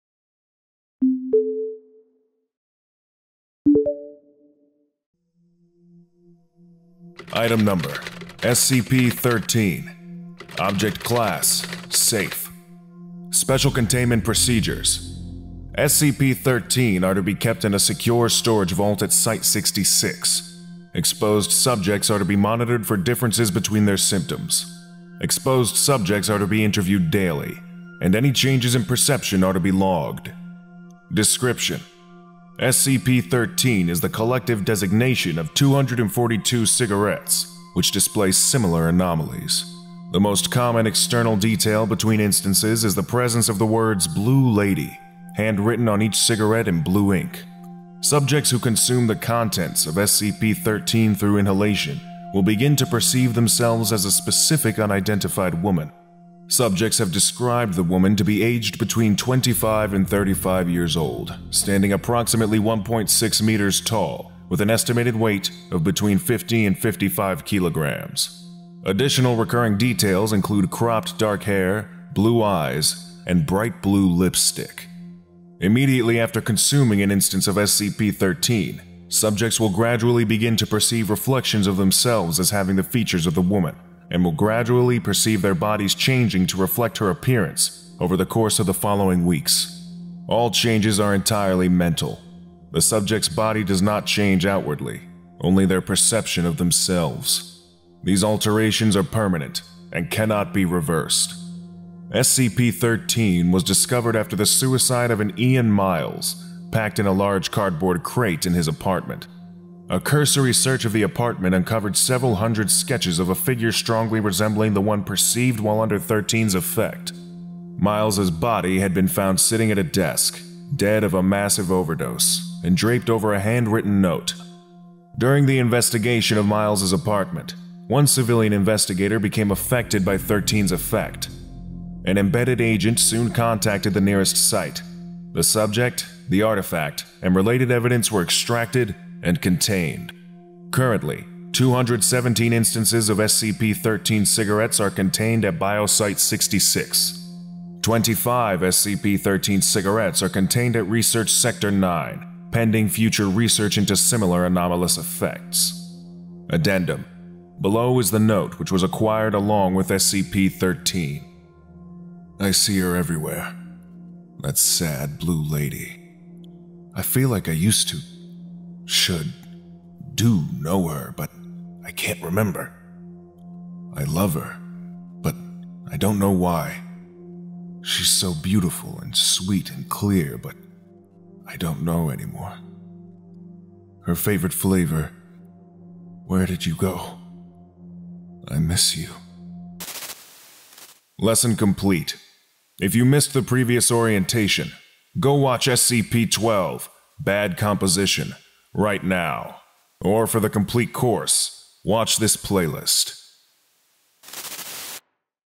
SCP-013 | Blue Lady Cigarettes (SCP Orientation)
Today we will be studying item number SCP-013: Blue Lady Cigarettes, Object class: Safe.

SCP-013 is the collective designation of 242 cigarettes which display similar anomalies. Immediately after consuming an instance of SCP-013, subjects will gradually perceive their bodies changing to reflect the appearance of an unidentified young woman over the course of the following weeks. All changes are entirely mental; the subject’s body does not change outwardly, only their perception of themselves.
These alterations are permanent, and cannot be reversed.

and released under Creative Commons Sharealike 3.0. Contributor: Dexanote

Artist:Laovaan